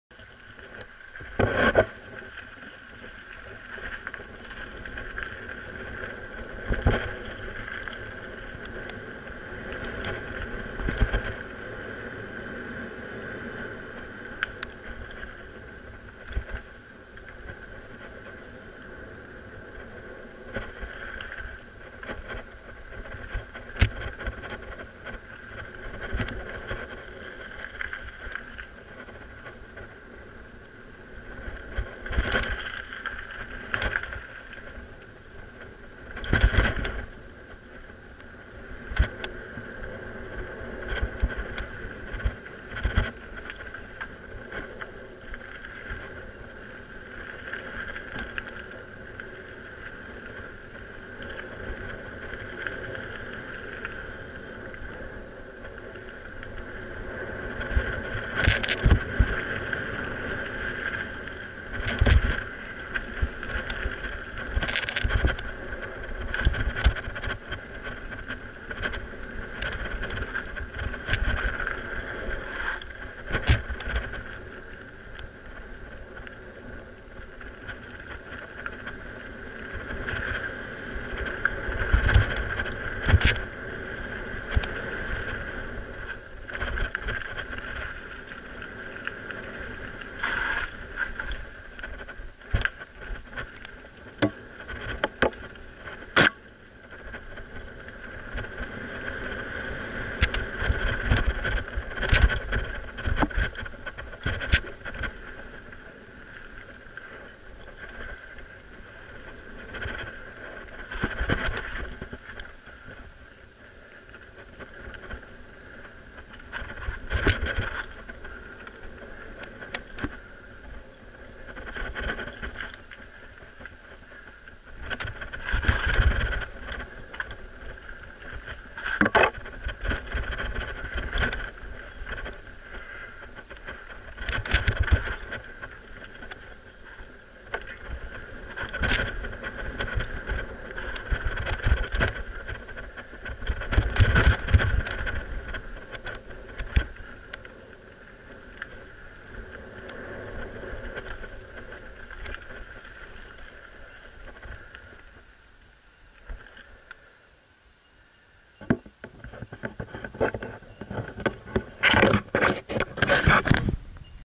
Mountain Biking .......
Video of my favorite section of mountain bike trail at the Glenrock State Conservation Area,NSW Australia..